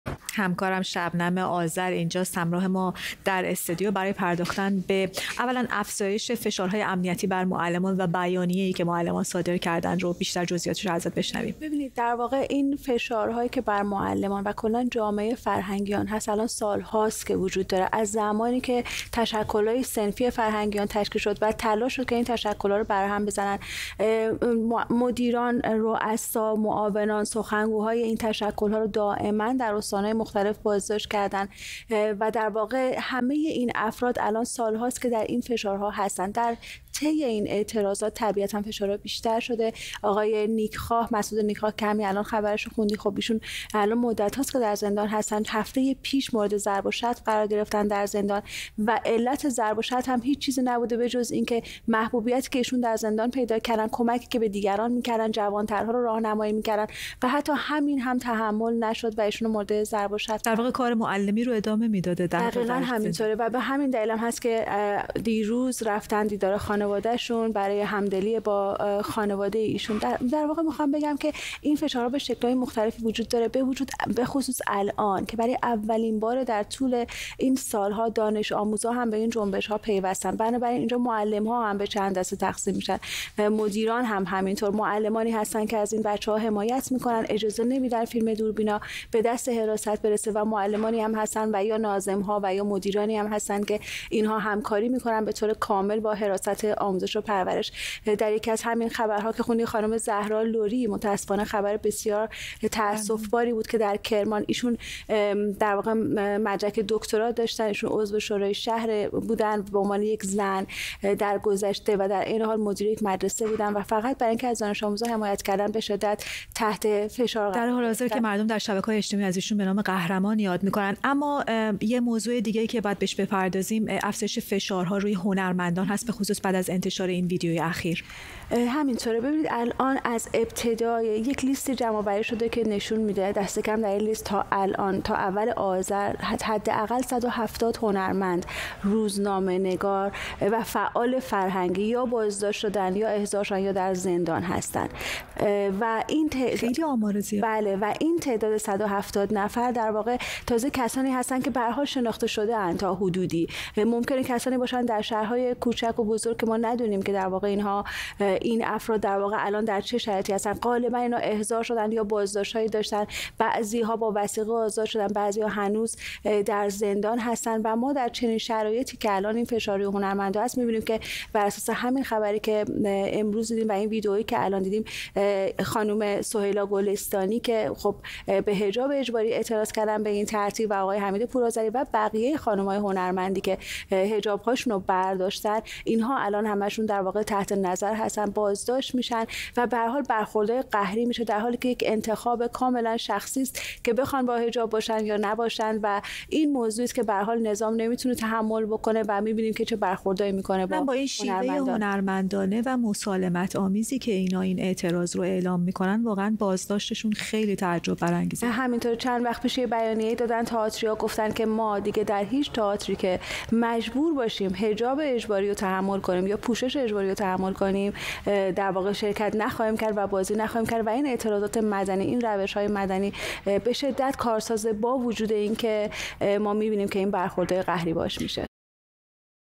فشار و سرکوب جمهوری اسلامی بر معلمان ایران و همچنین وضعیت معلمان زندانی
شبم آذر، خبرنگار ایران اینترنشنال از فشار و سرکوب جمهوری اسلامی بر معلمان ایران و همچنین وضعیت معلمان زندانی می‌گوید